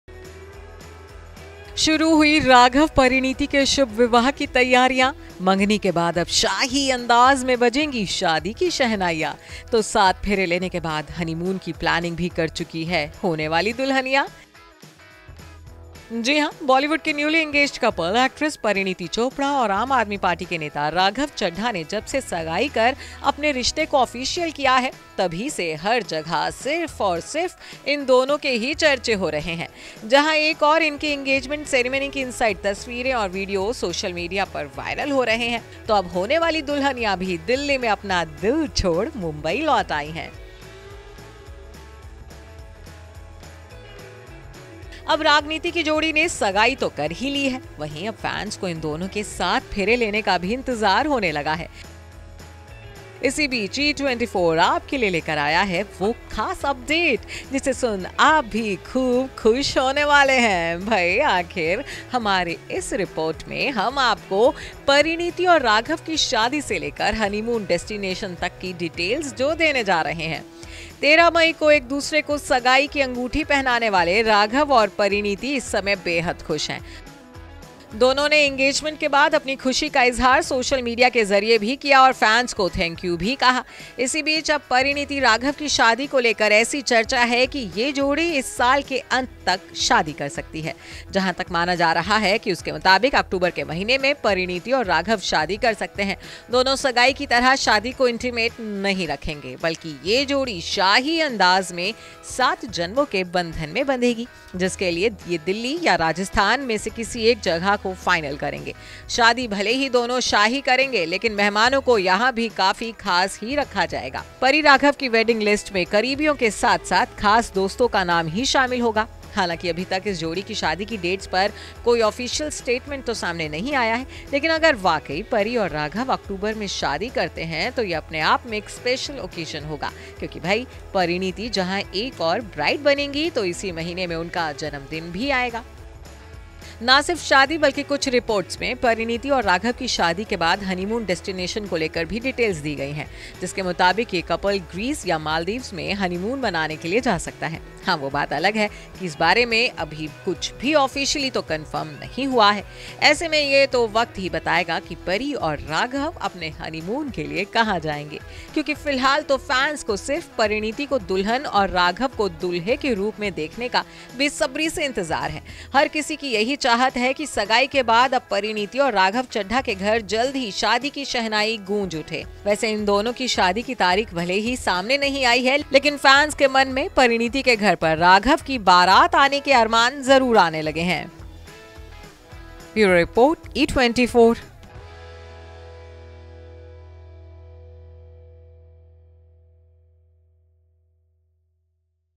शाही अंदाज़ में Raghav Chadha संग सात फेरे लेंगी Parineeti Chopra, हनीमून के लिए भी फाइनल की जगह
अब 'रागनीति' की जोड़ी ने सगाई तो कर ही ली है..वहीं, अब फैन्स को इन दोनों के सात फेरे लेने का भी इंतजार होने लगा है..इसी बीच, E24 आपके लिए ले आया है..वो खास अपडेट..जिसे सुन आप भी खूब खुश होने वाले हैं..भई, आखिर हमारी इस रिपोर्ट में हम आपको परिणीति-राघव की शादी से लेकर हनीमून डेस्टिनेशन तक की डिटेल्स जो देने जा रहे हैं।